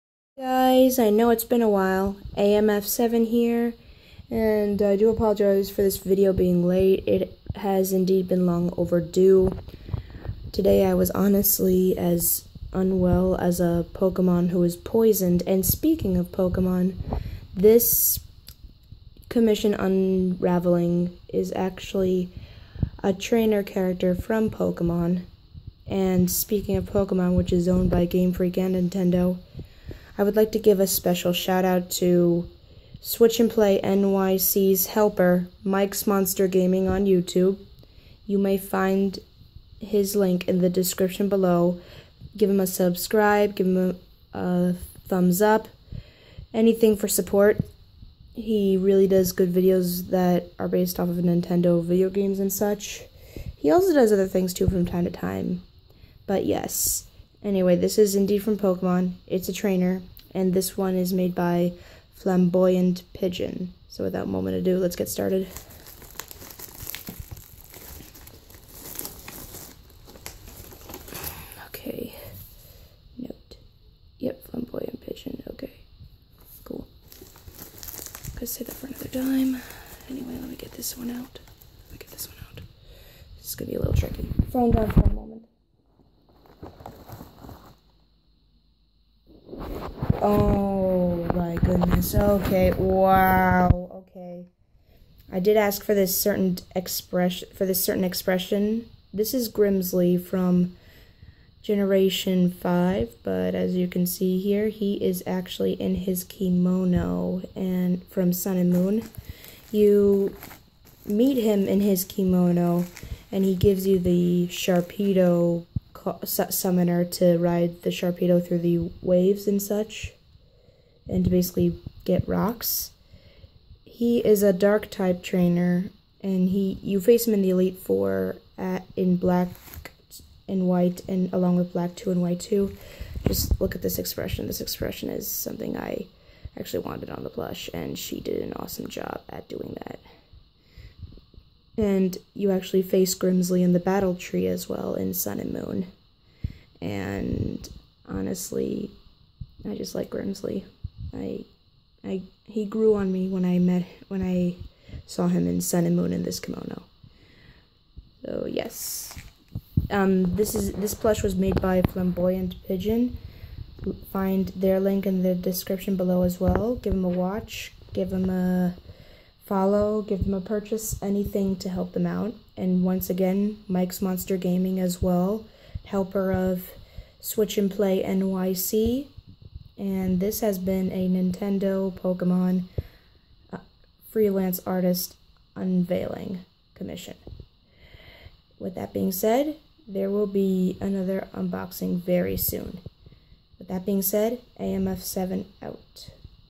Pokemon Unboxing, Special Nintendo Community Member Shout Out
Unova Elite 4 Trainer Grimsley in his Sun and Moon Kimono unveiling.

Special shout out goes to MMG Mike's Monster Gaming! Checkout their channel here! give them a like, a follow, anything that helps them out! They list their other sites as well in their about page!

give them a follow, commission, anything to help them out

Plush made by Flamboyant Pigeon